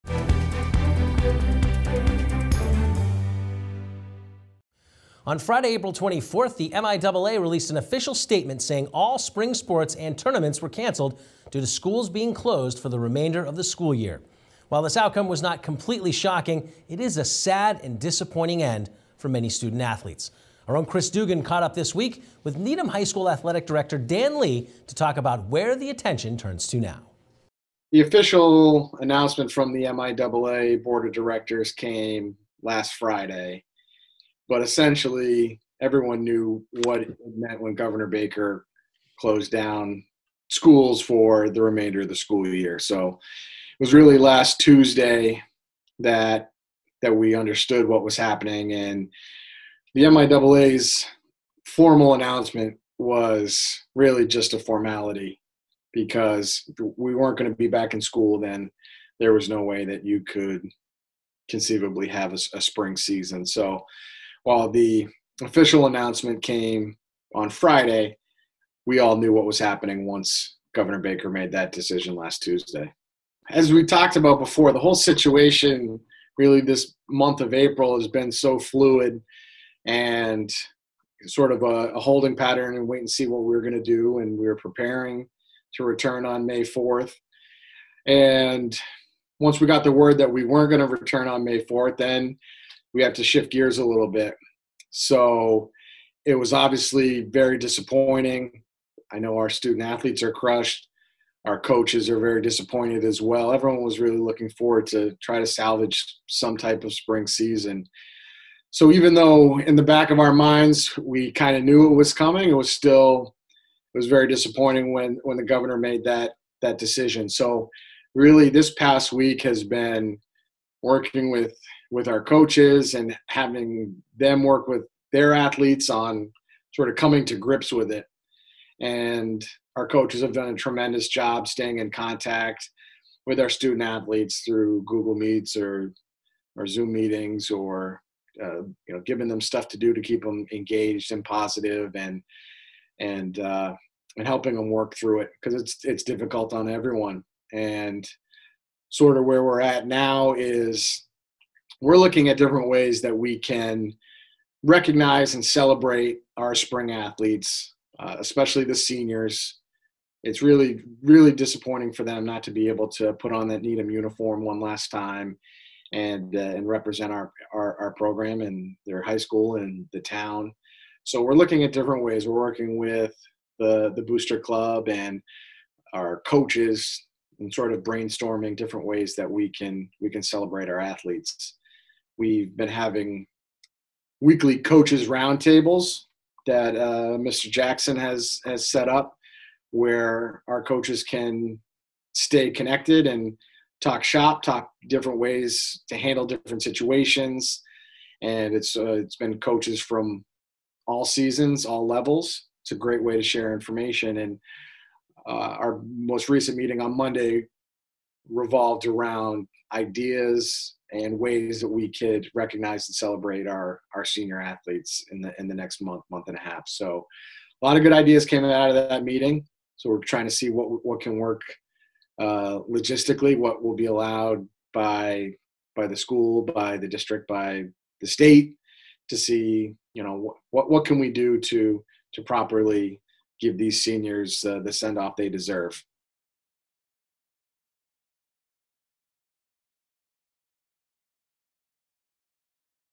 Spring Season Cancellation
Needham High School Athletic Director, Dan Lee, discusses the cancellation of the spring season since schools are closed because of COVID-19.